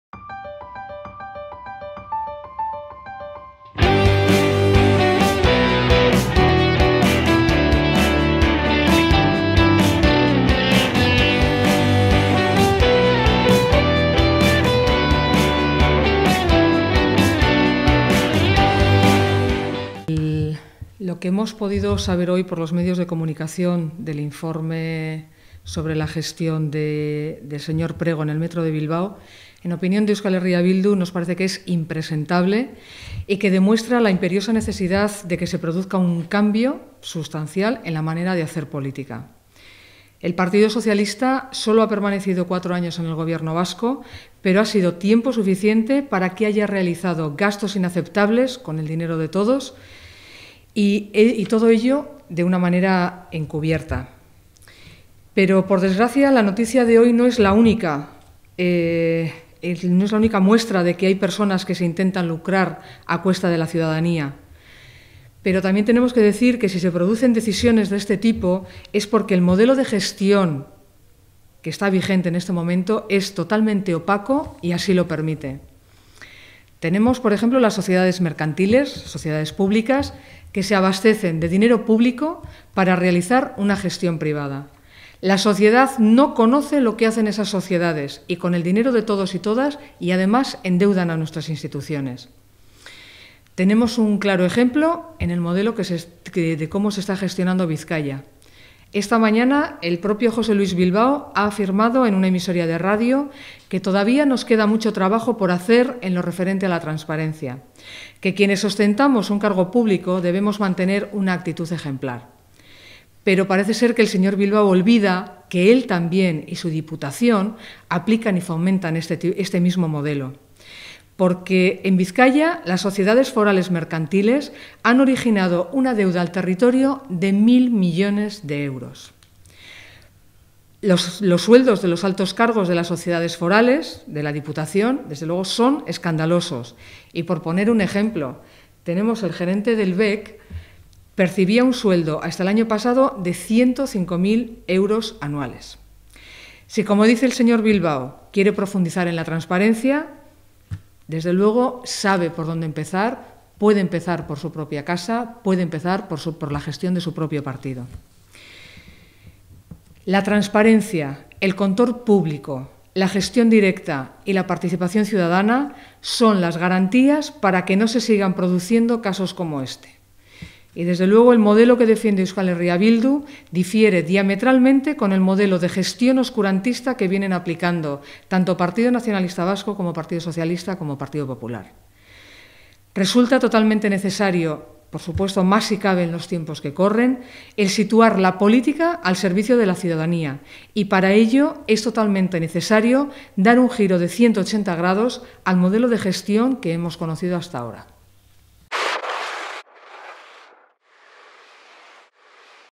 El modelo de gestión opaca que propicia casos como el de Prego es el mismo que rige en Bizkaia
Rueda de prensa de EH Bildu sobre los datos de gastos y gestión de Metro Bilbao dados a conocer hoy en los medios de comunicación. La concejal del Ayuntamiento de Bilbao Helena Gartzia a leído el texto en castellano.